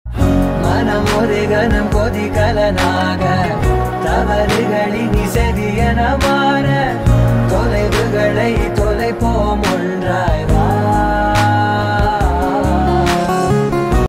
VARKALA | LOVE | OUTDOOR SHOOT | THE PHOTOTODAY
This couple started spreading wings flying on the songs of their heart strings! 'Love' perpectually stuck between them,tugging their heart strings.Time for some romantic moments together!